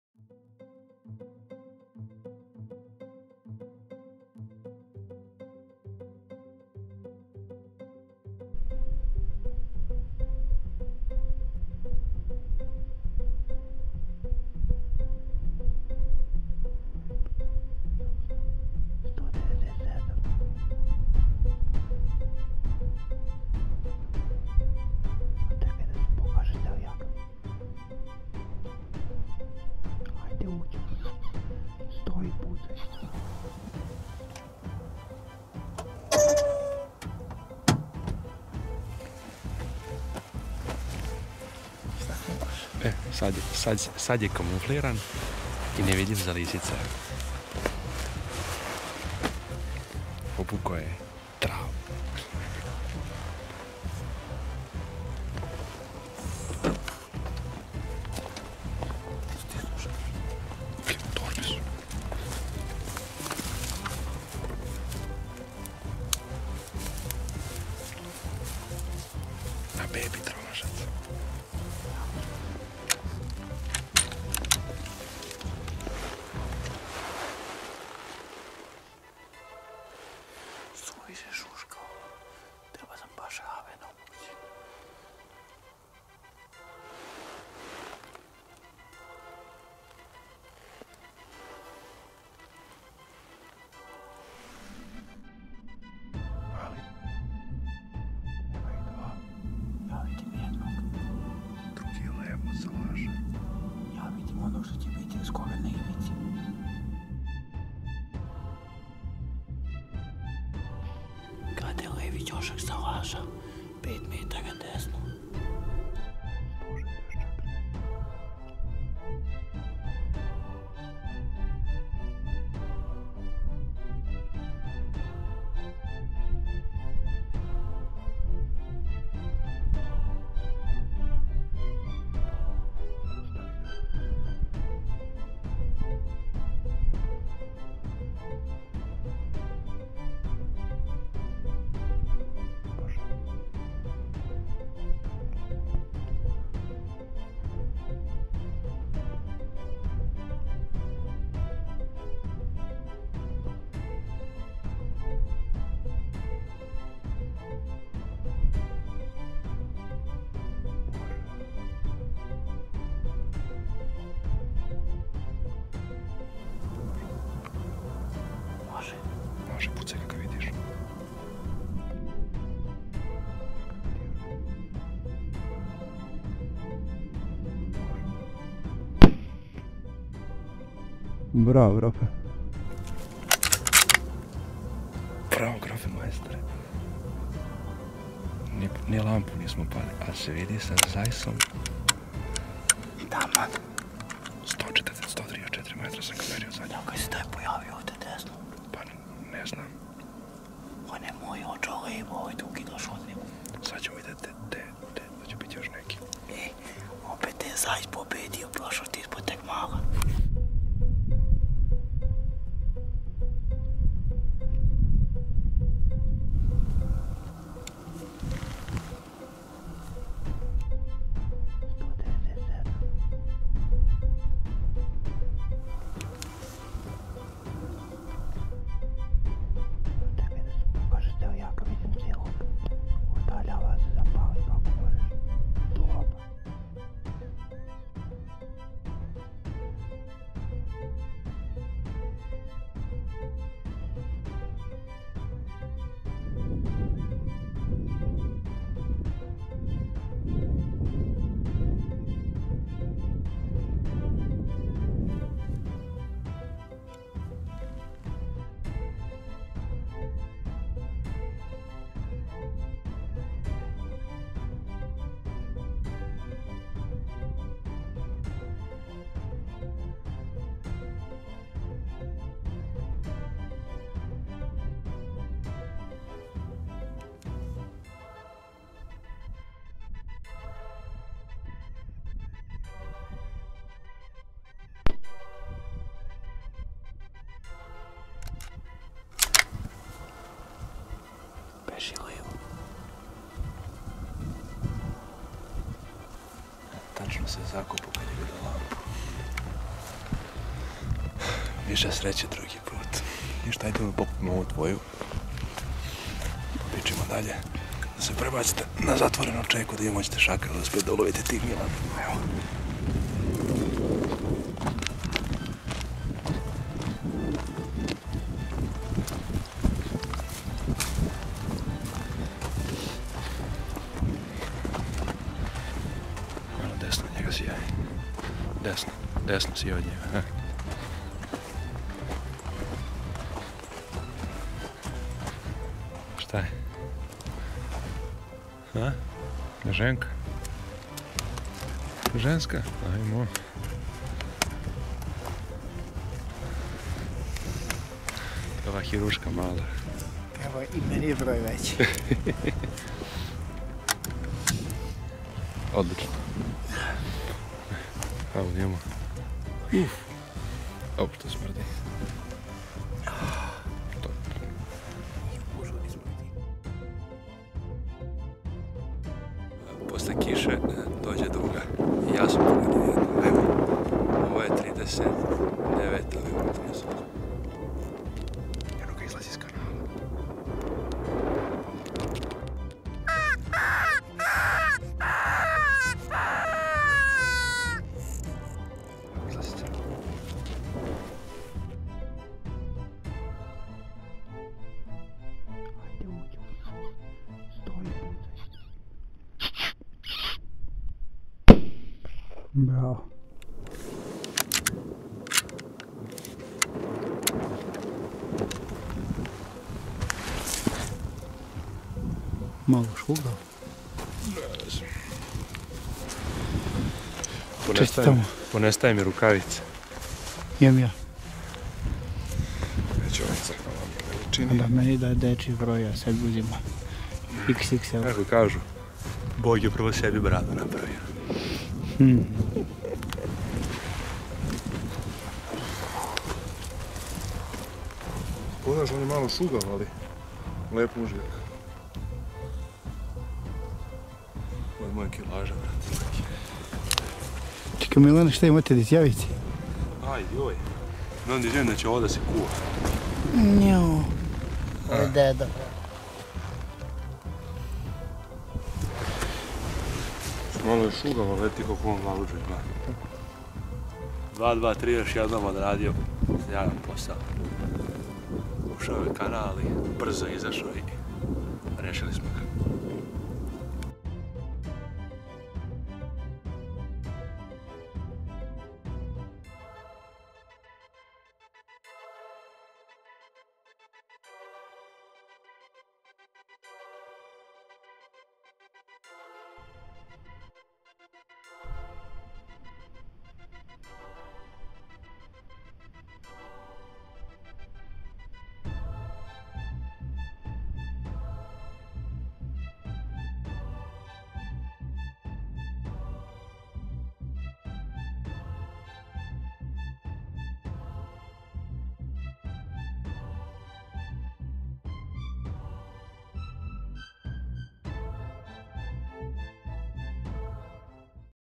E76 Lov na lisice sa 223 rem/Fox hunting wtih 223 rem.
Ove nedelje jedan izlazak pun aktivnosti, lovili smo čekanjem. Neke od lisica smo pucali i za dana dok ostale po mraku. Naravno kalibar 223 rem i V-max su odradili odličan posao.
Do sledećeg utorka u 19:00 Dobar pogled.

Wild Srbija majice poručite na našim stranicama na društvenim mrežama.

Dobar pogled, do slećeg utorka!